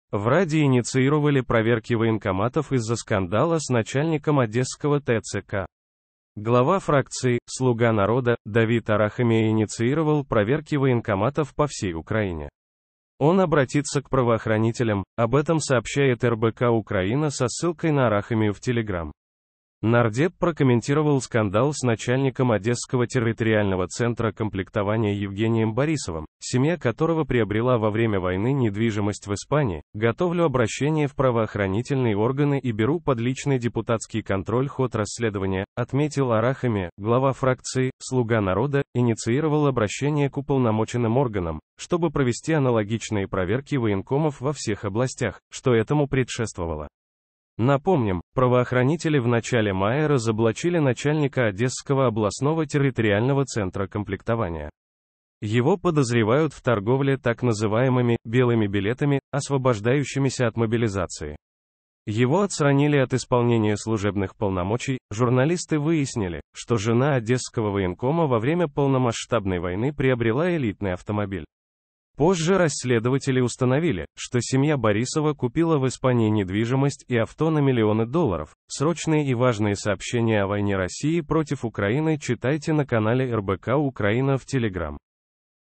В Раде инициировали проверки военкоматов из-за скандала с начальником одесского ТЦК.
ДавидАрахамия Слуганарода Глава фракции "Слуга народа" Давид Арахамия инициировал проверки военкоматов по всей Украине. Он обратится к правоохранителям. Об этом сообщает РБК-Украина со ссылкой на Арахамию в Telegram. Нардеп прокомментировал скандал с начальником одесского территориального центра комплектования Евгением Борисовым, семья которого приобрела во время войны недвижимость в Испании. "Готовлю обращение в правоохранительные органы и беру под личный депутатский контроль ход расследования", - отметил Арахамия. Глава ф